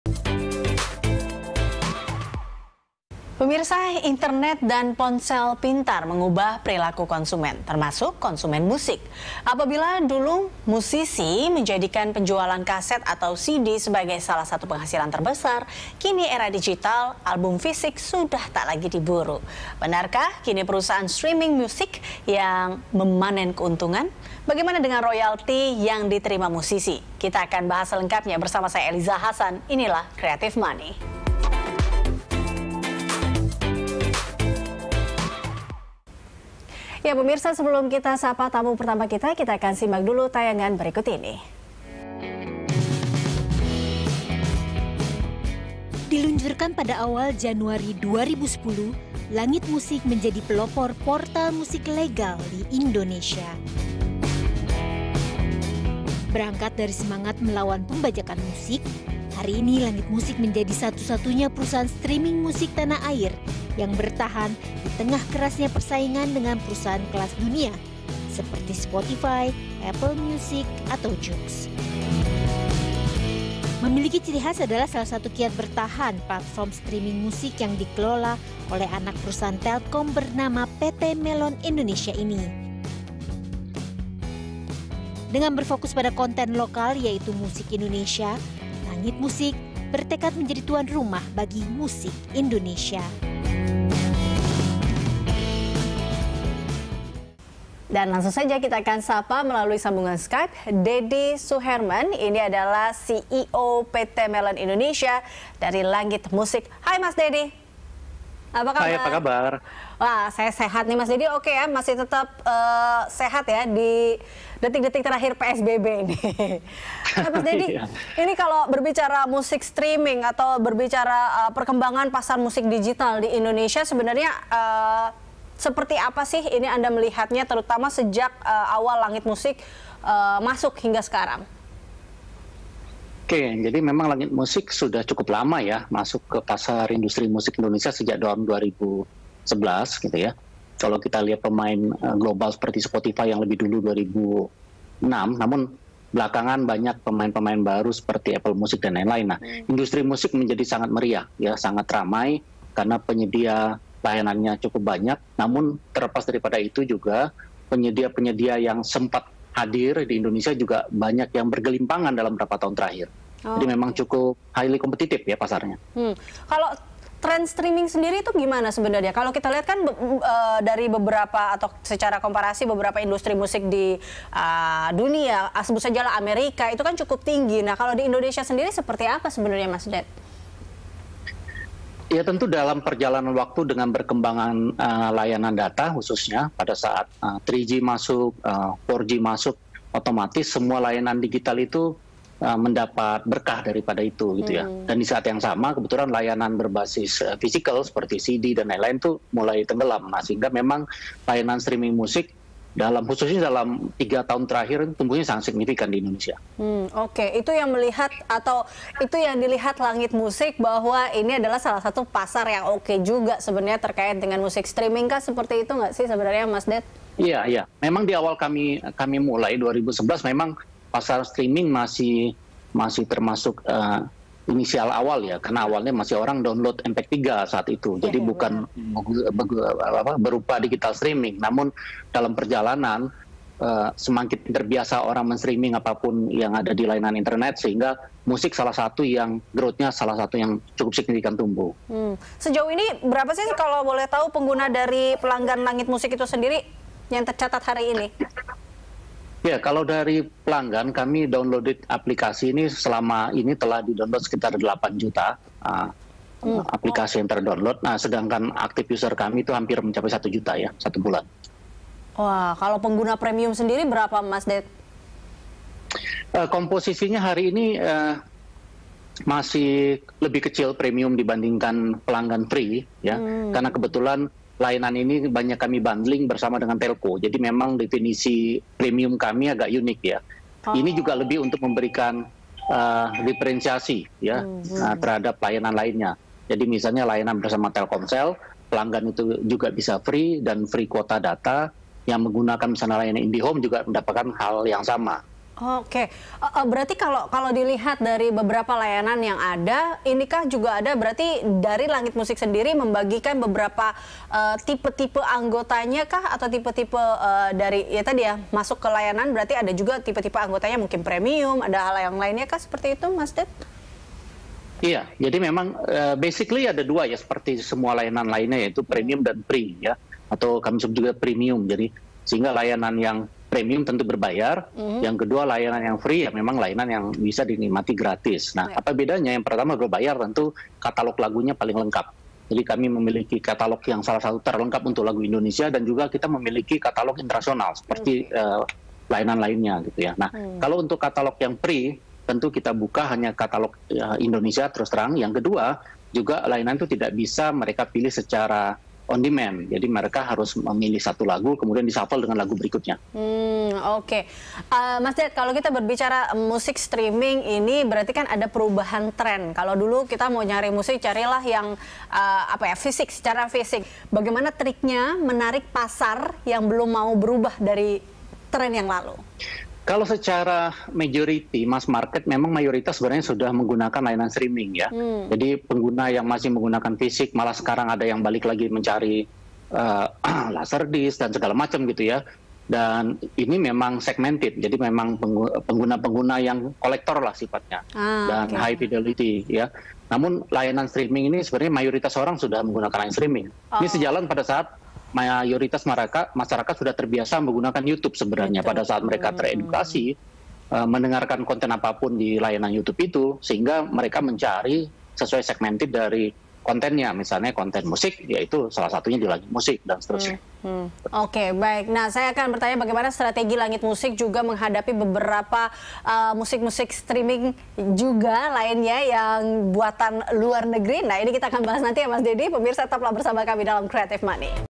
Creative Money: Mengail Cuan di Musik Streaming #1
Apabila dulu musisi menjadikan penjualan kaset atau CD sebagai salah satu penghasilan terbesar. Namun di era digital sekarang ini album fisik sudah tak lagi diburu.